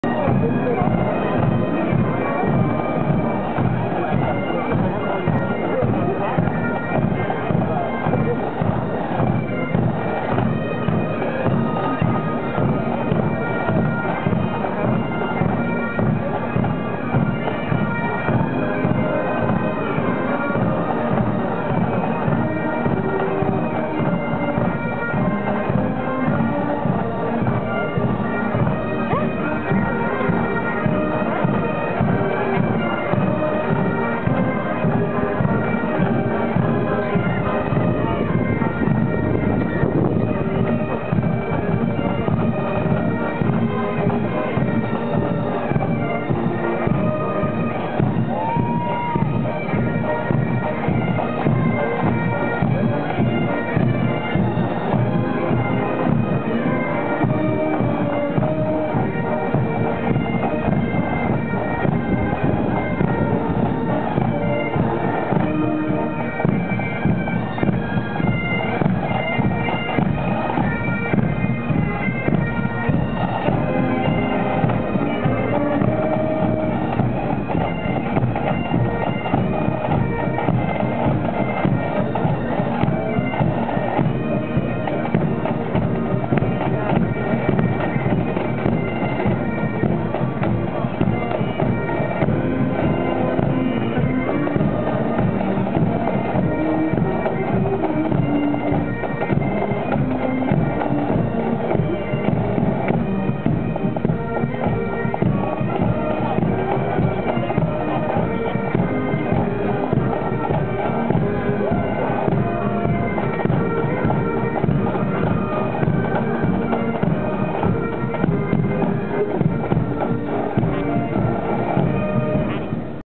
RMC Sunset Ceremony '09 - Pipes and Drums (1of2)
Following Convocation Ceremonies at The Royal Military College of Canada (RMC), a Sunset Ceremony is held to showcase a wide arrange of military skills, and teams and clubs, including the pipes and drums band.